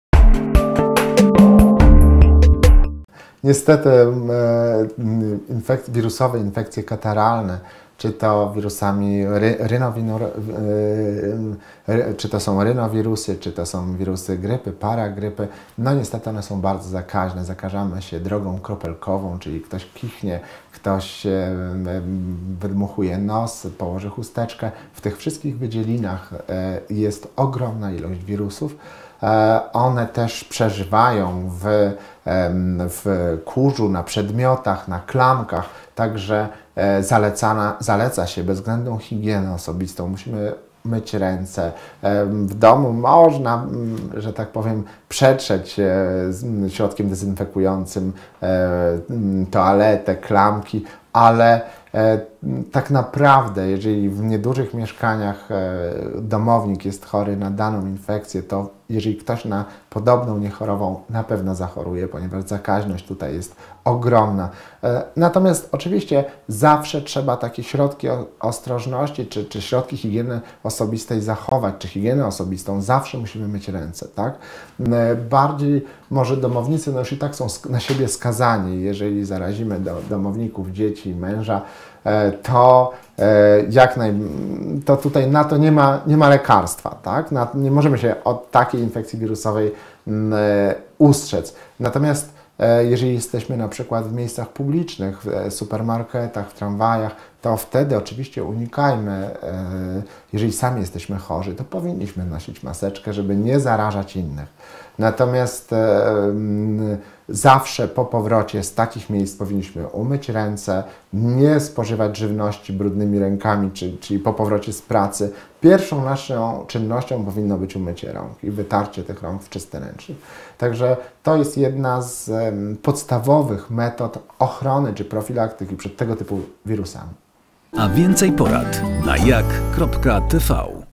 dr P. Gryglas: Jak chorując nie zarazić domowników? | WIRUSY | INFEKCJE
Wirusy, infekcje atakują. Jak samemu chorując nie zarazić domowników? Czy to możliwe? Wyjaśnia doktor nauk medycznych Piotr Gryglas